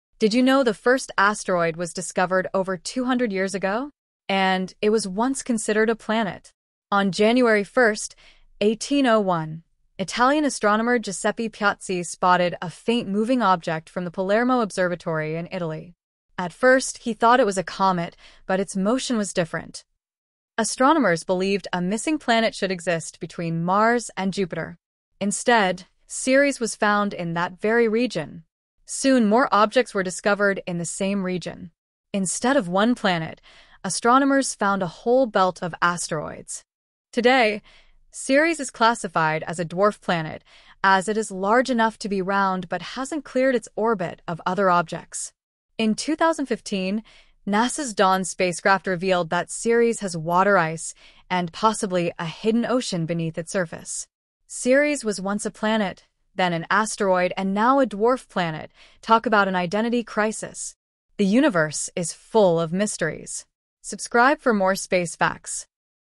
The First Asteroid Ever Discovered!
Did you know the first asteroid ever discovered was once considered a planet? 🤯 Meet Ceres, the dwarf planet that changed our understanding of the solar system! Discovered on 1st January 1801 by the Italian astronomer Giuseppe Piazzi, Ceres was the first object found in the region between Mars and Jupiter, known today as the asteroid belt. 🌌 Initially thought to be a comet, it soon became clear that Ceres was something entirely different — an asteroid!

Ceres' discovery sparked the search for other small bodies in this region, leading to the identification of other asteroids like Pallas, Juno, and Vesta. This led astronomers to realise that instead of one missing planet, there was an entire asteroid belt full of smaller celestial bodies.

But Ceres is not just any asteroid. It’s a dwarf planet! With a diameter of 940 km, Ceres is the largest object in the asteroid belt. But that’s not all — recent space missions, like NASA’s Dawn spacecraft, have revealed that Ceres has water ice, and it might even have a hidden ocean beneath its surface! 🌊💧

In this video, we take a quick journey through the history of Ceres' discovery, its importance to science, and its modern-day significance. From being a planet to a dwarf planet, Ceres has an incredible story that continues to intrigue astronomers and space enthusiasts alike.

Join us as we explore how Ceres helped unlock the mysteries of the solar system! 🚀✨